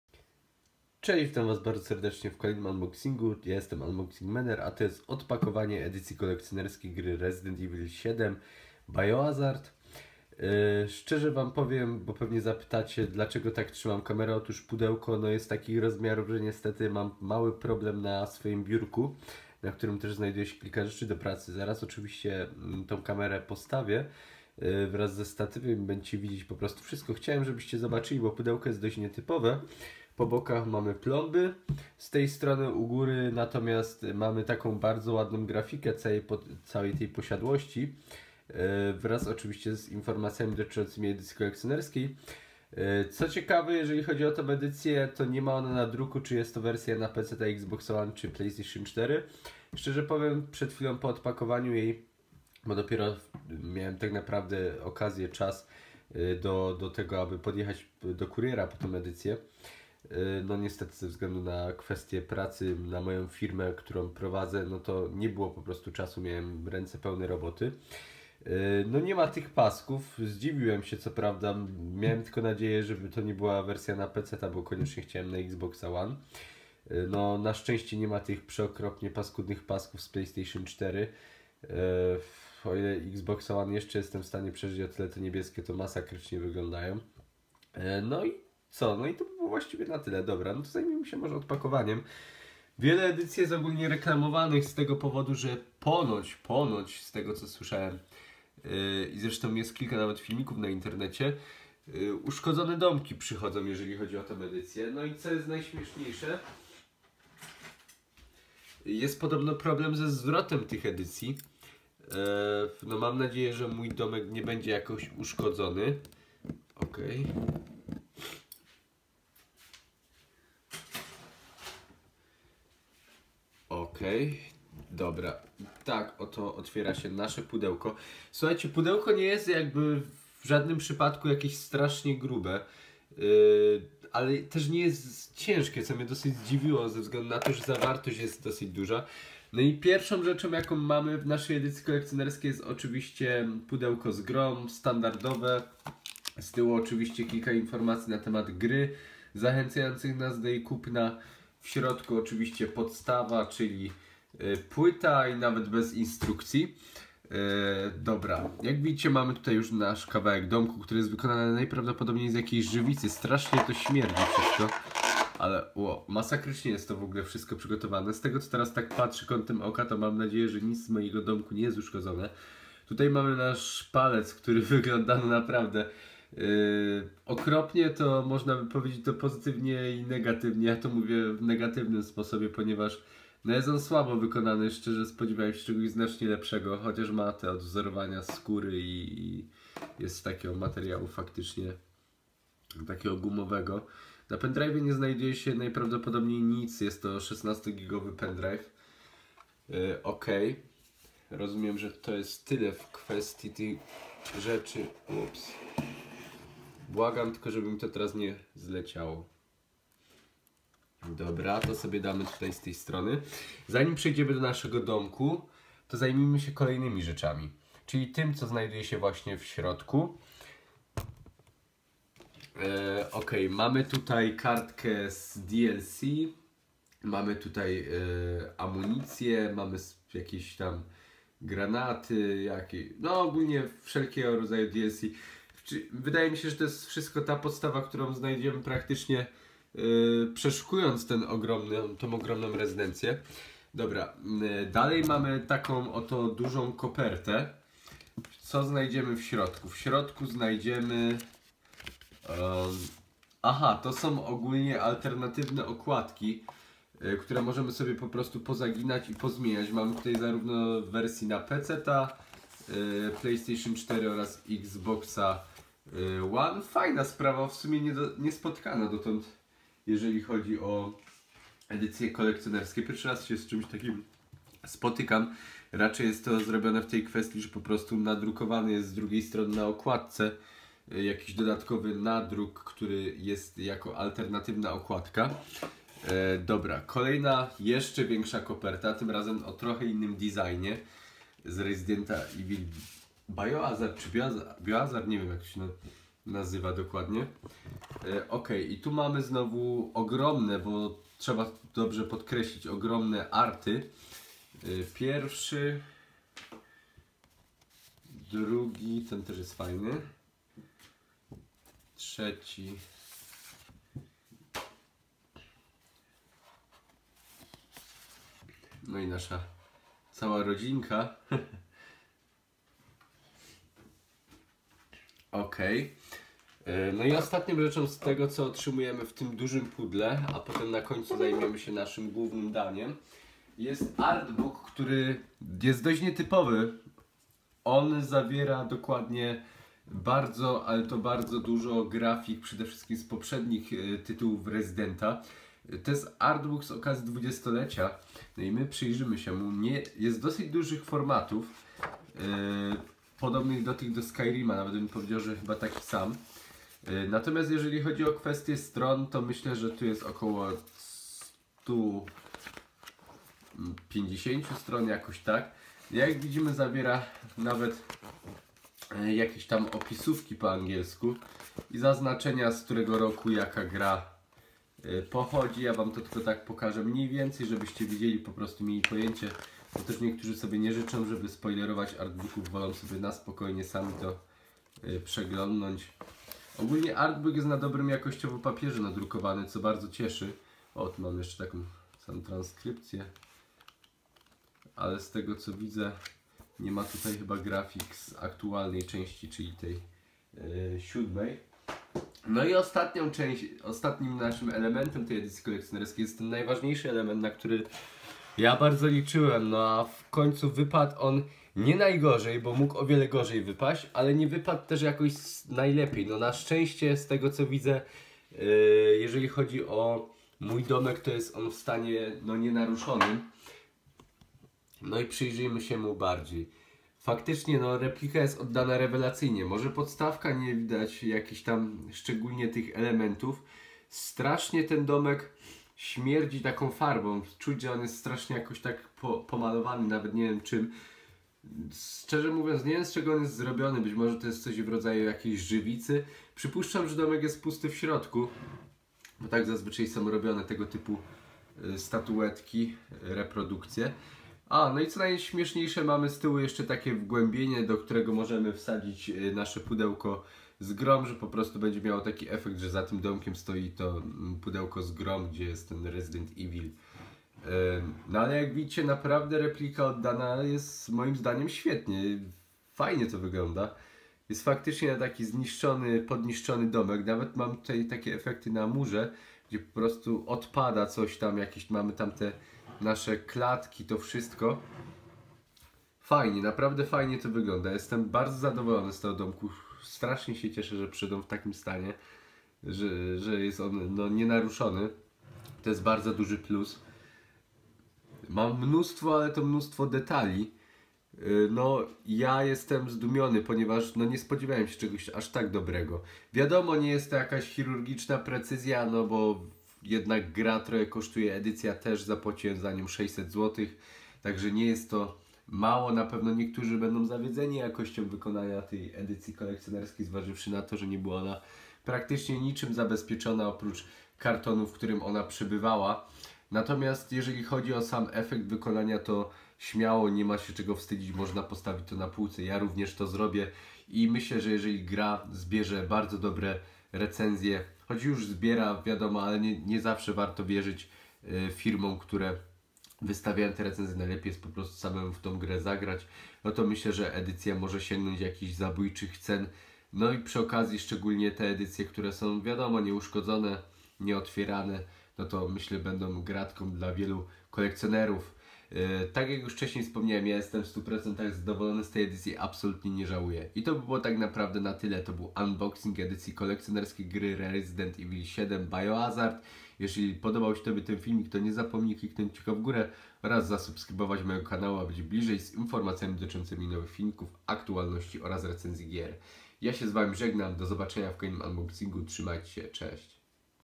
Resident Evil VII Edycja Kolekcjonerska Unboxing/Rozpakowanie
W skład Edycji kolekcjonerskiej wchodzą:

-Gra Resident Evil 7 biohazard w polskiej wersji językowej na wybraną platformę sprzętową
-Szczegółowo odwzorowana replika posiadłości rodziny Bakerów o wysokości 18 cm
-Artbook na 20-lecie serii Resident Evil
-USB w kształcie palca o pojemności 16 GB
-5 litografii przedstawiających grafiki z gry Resident Evil 7 biohazard
-Alternatywna okładka gry
-DLC z pakietem survivalowym, który zawierać będzie liczne użyteczne przedmioty, tajemniczą szczęśliwą monetę i odblokowany poziom trudności „Madhouse”
-Kolekcjonerskie pudełko

Podobał Ci się ten filmik ? Nie zapomnij kliknąć kciuka w górę !